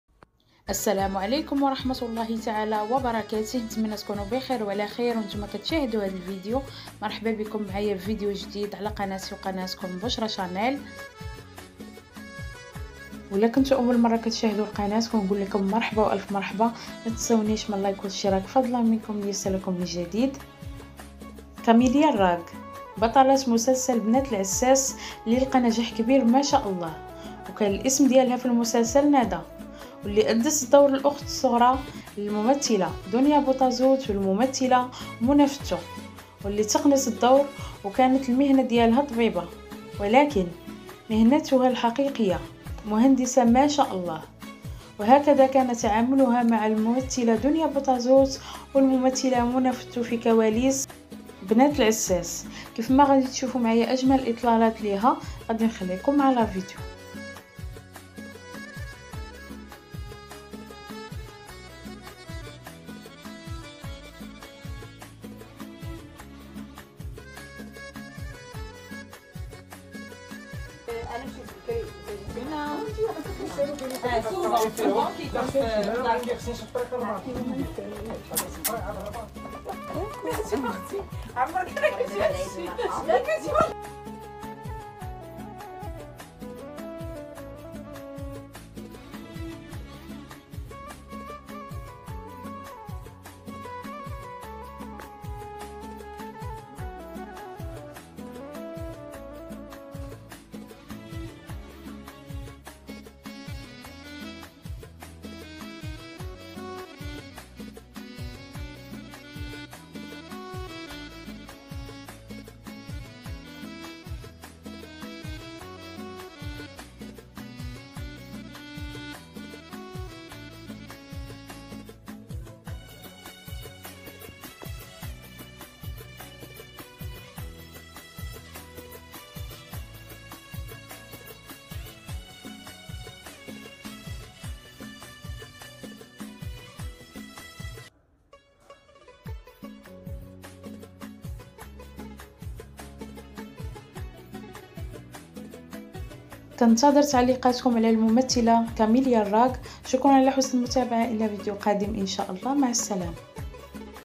مهنتها الحقيقية مهندسة...تعرفوا على بطلة بنات العساس وكيف كان تعاملها مع دنيا بوطازوت ومنى فتو
شاهدوا لأول مرة مهنتها الحقيقية وكيف كان تعاملها مع دنيا بوطازوت ومنى فتو في الكواليس بنات العساس